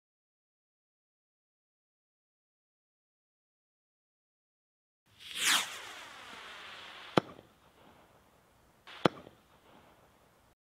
Yellow Star Rakete Nico
Kleine Rakete mit kleinem Bouquet, aber mit intensivem Gelb und darum gefällt mir die Rakete.